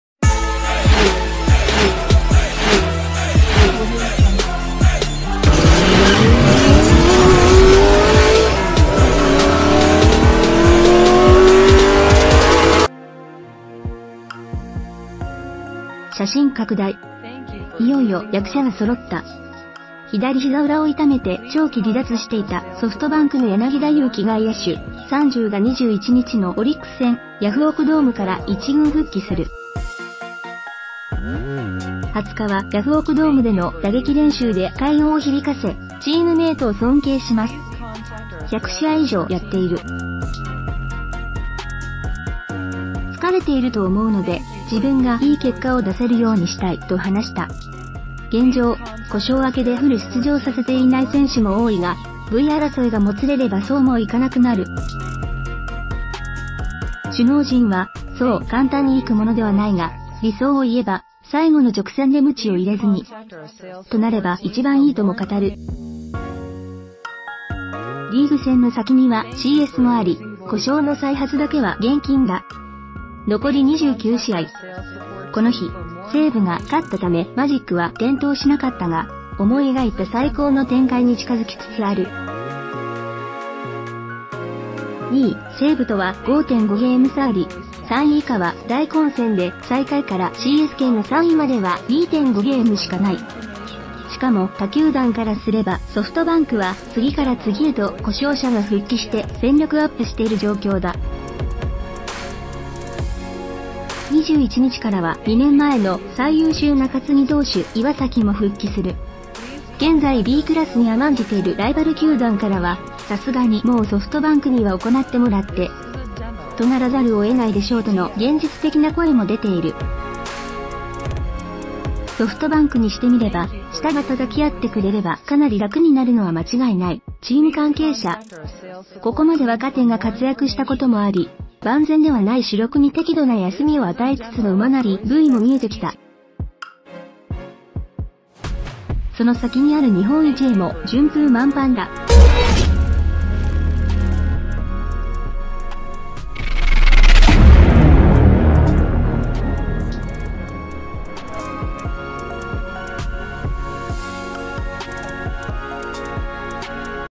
いよいよ柳田一軍復帰！ 見えたソフトバンクの#8221;馬なり#8221;ＶTop News
,見えたソフトバンクの#8221;馬なり#8221;ＶTop,News,いよいよ柳田一軍復帰！
いよいよ柳田一軍復帰！左ヒザ裏を痛めて長期離脱していたソフトバンクの柳田悠岐外野手（３０）が２１日のオリックス戦（ヤフオクドーム）から一軍復帰する。２０日はヤフオクドームでの打撃練習で快